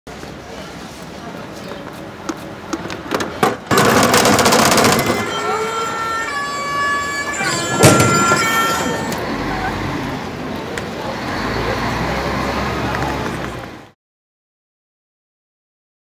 Les bouteilles à boissons en PET vide permettent de produire de nouvelles bouteilles d'HENNIEZ.
Une belle surprise pour ceux qui éliminent correctement leur bouteille en PET vide!